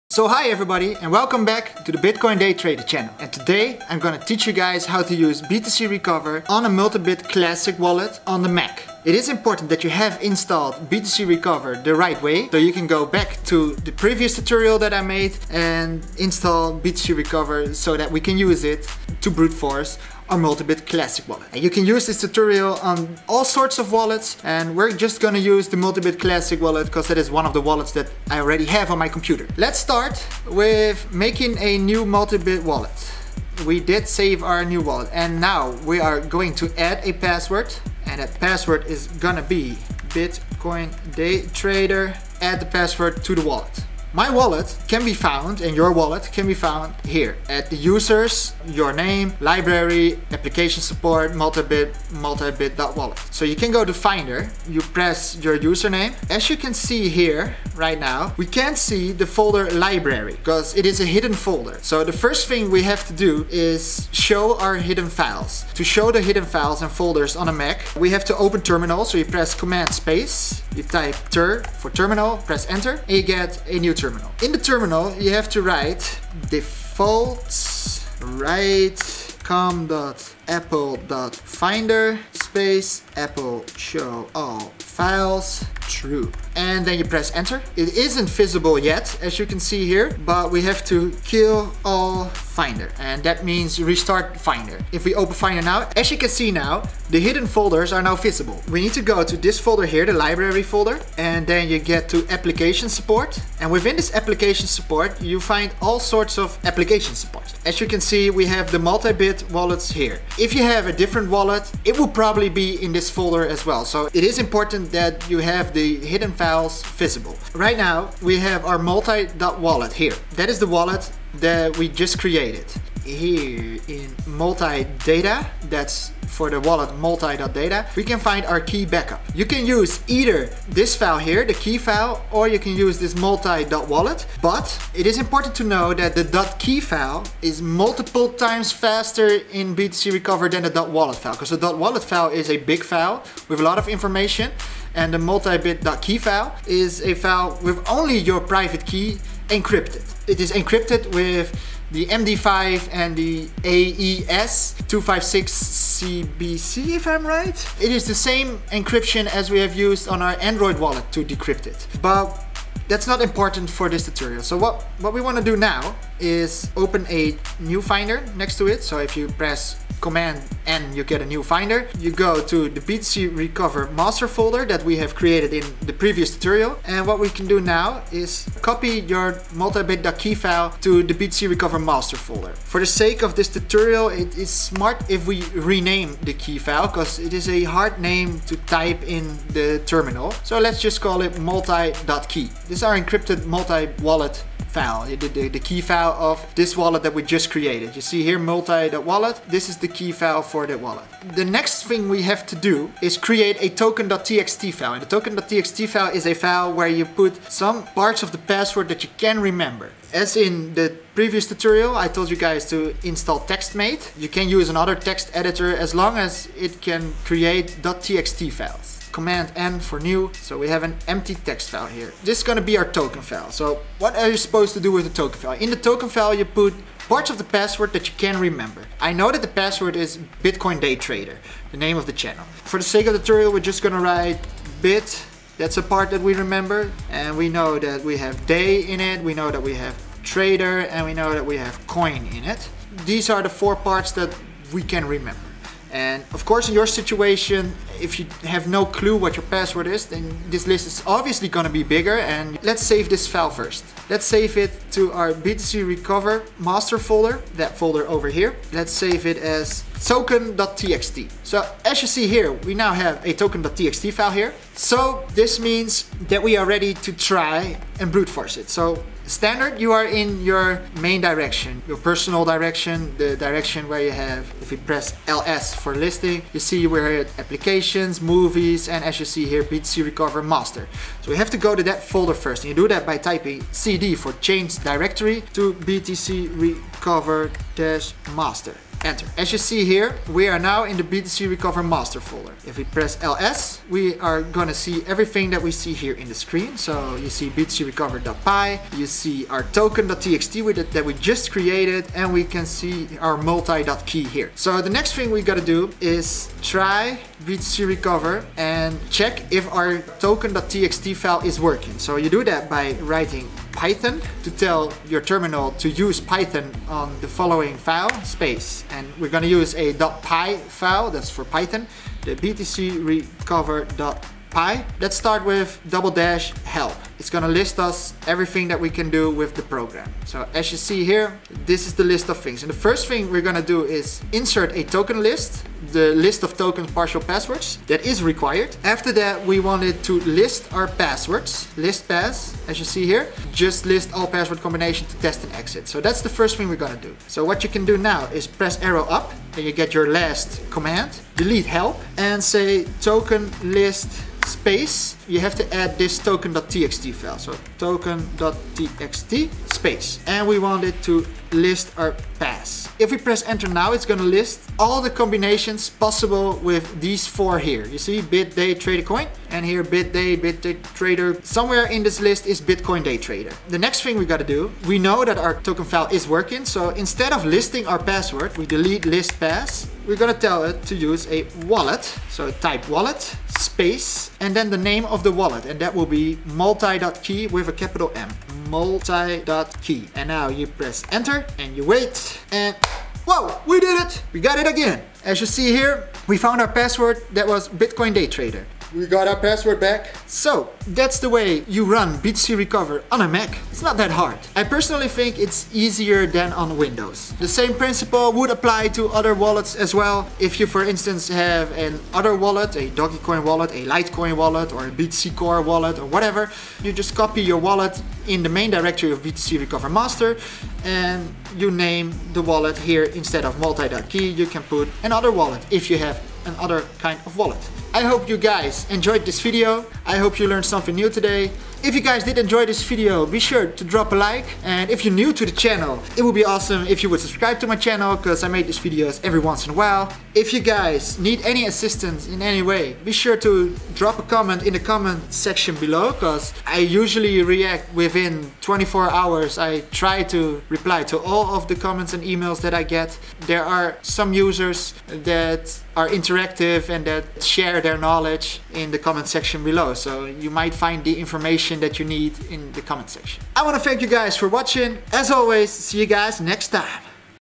Brute-force your Multibit Classic wallet on mac OSX with btcrecover.
Show hidden files on mac - type in terminal:
defaults write com.apple.Finder AppleShowAllFiles true
killall Finder

Hide hidden files on mac - type in terminal:
defaults write com.apple.Finder AppleShowAllFiles false
killall Finder